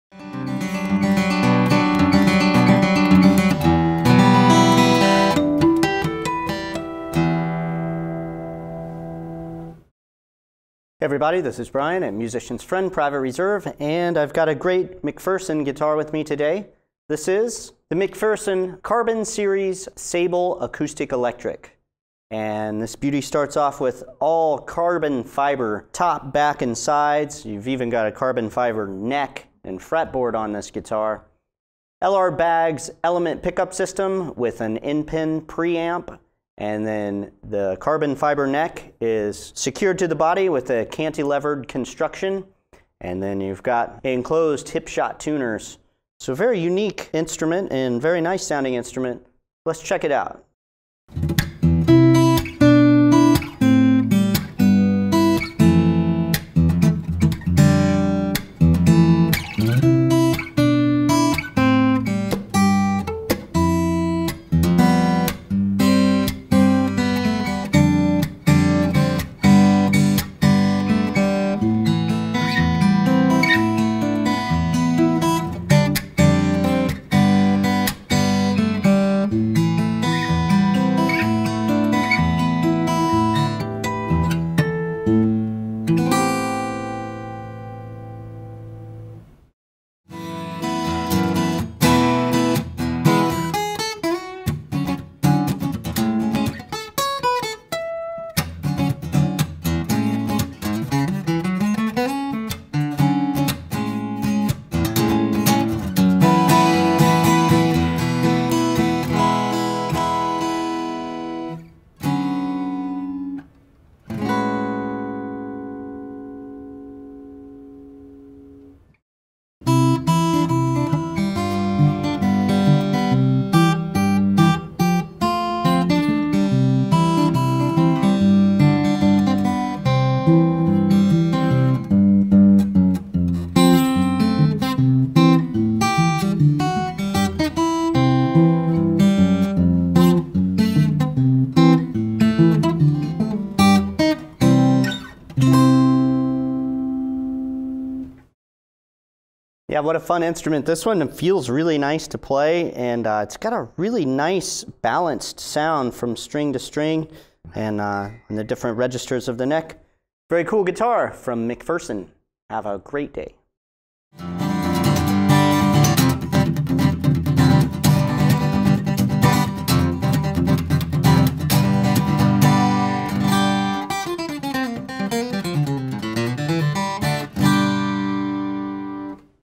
McPherson Carbon Series Sable Acoustic-Electric Guitar
Speak to a Private Reserve guitar advisor: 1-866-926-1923

This guitar was played out of the box with factory strings and with no additional set-up. The track was finished without any kind of EQ or compression.

Serial# CF296, Weight: 5.1 lbs. / 2.31 Kg.

Mics: (2) Shure SM81s → → Universal Audio 2-610 Silverface Dual Channel Mic Pre

Recorded at Musician’s Friend in October 2017.